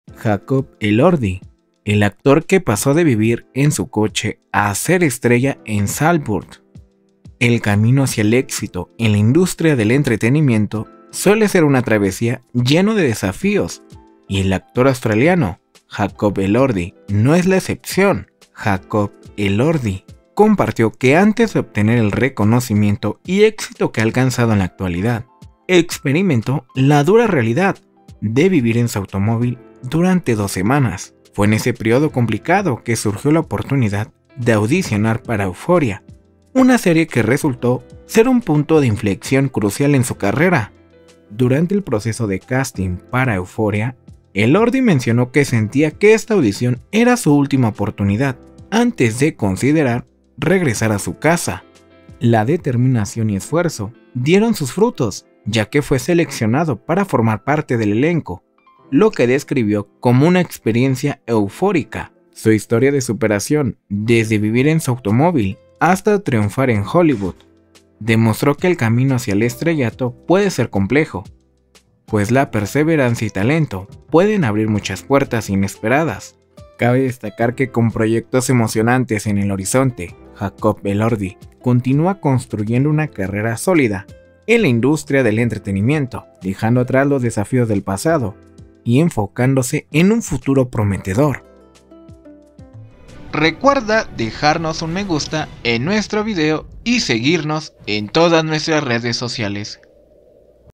Jacob Elordi pasó de vivir en su coche a ser estrella en Saltburn
JacobElordi pasó de vivir en su coche a ser estrella en Saltburn. 👏🎬

El camino hacia el éxito en la industria del entretenimiento suele ser una travesía llena de desafíos, y el actor australiano Jacob Elordi no es la excepción.